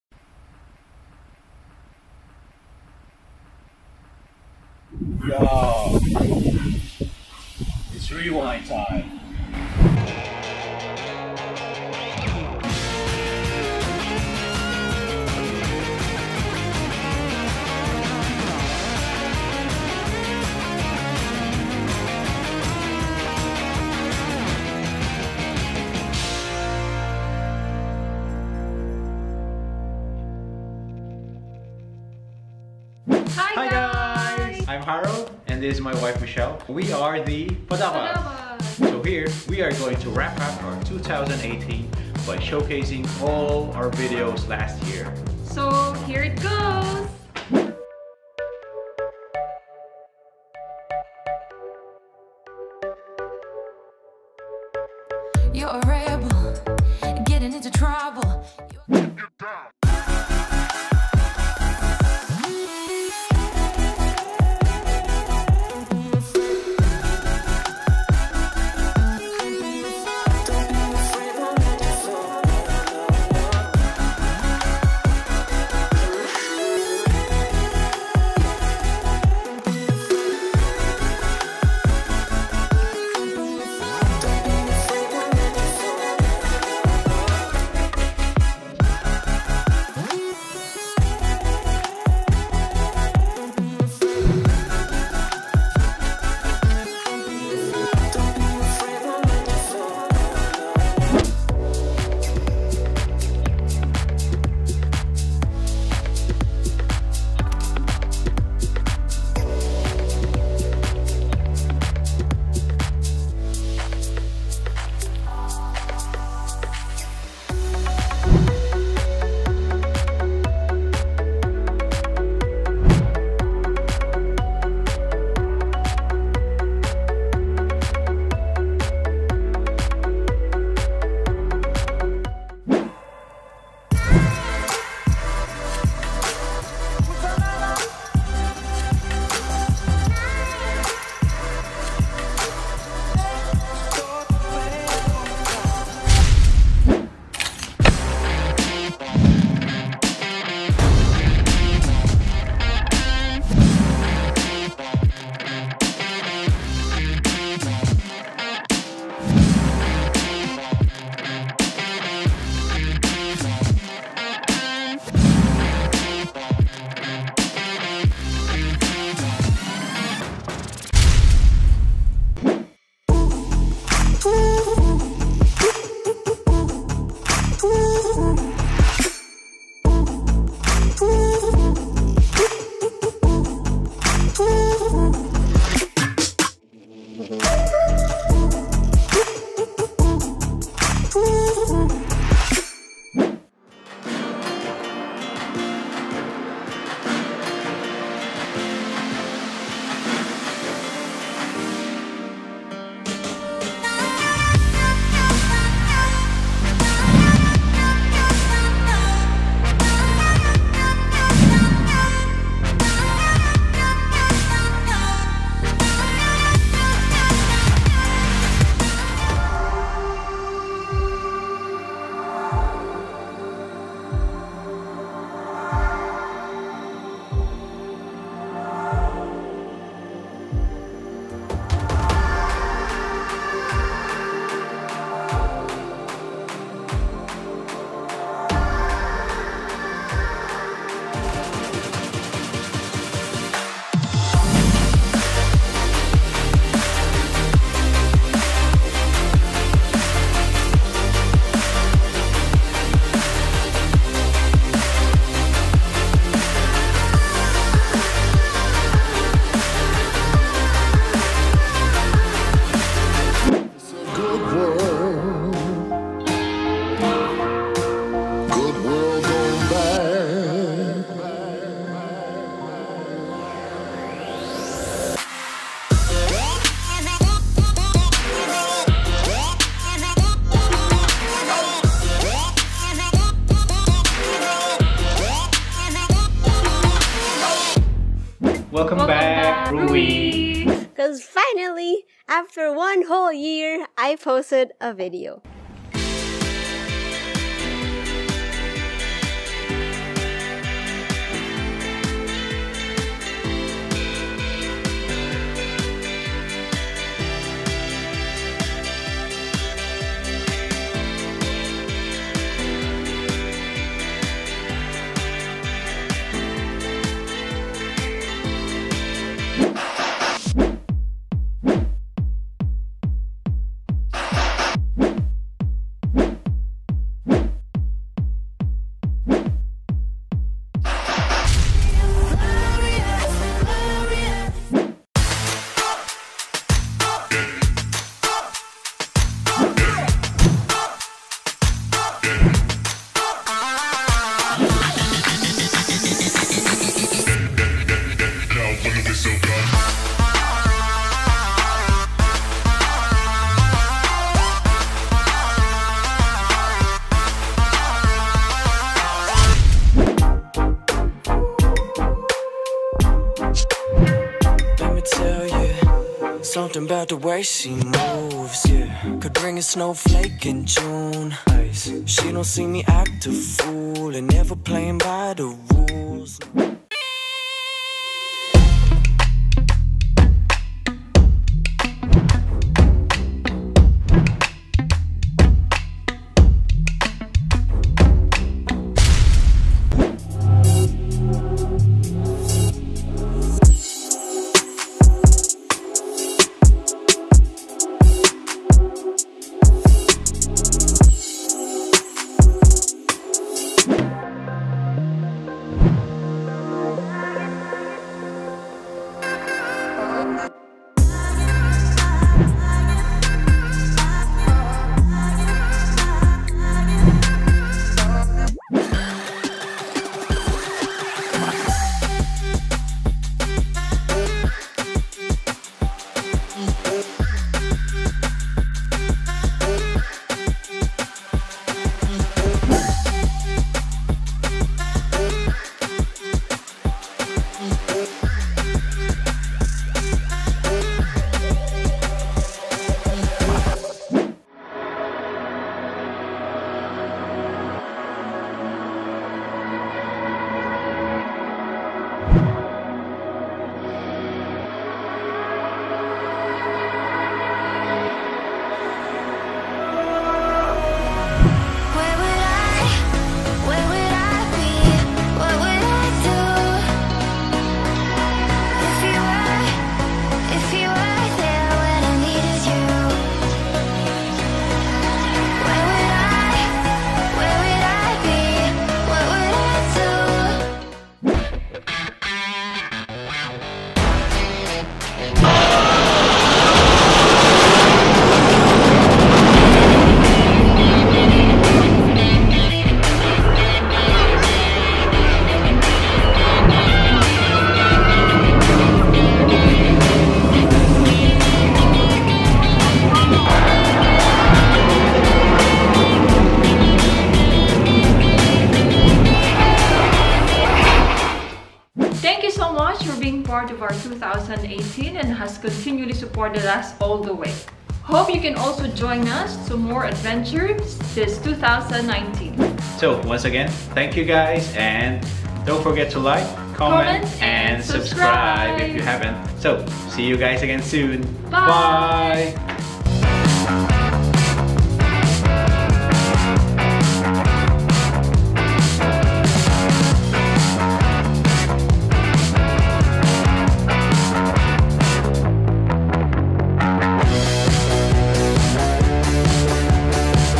The Padabas Rewind 2018
Before we start on our new journey to 2019, we will take you back to all our highlights in 2018.

Gear:
1. Canon G7x Mark ii
2. DJI Osmo x3
3. DJI Spark

Definitions:
1. Padaba - means Love in Bicol language of the Philippines
2. Bicol - The Bikol languages are a group of Central Philippine languages spoken mostly on the Bicol Peninsula in the island of Luzon, the neighbouring island province of Catanduanes and the island of Burias of Masbate."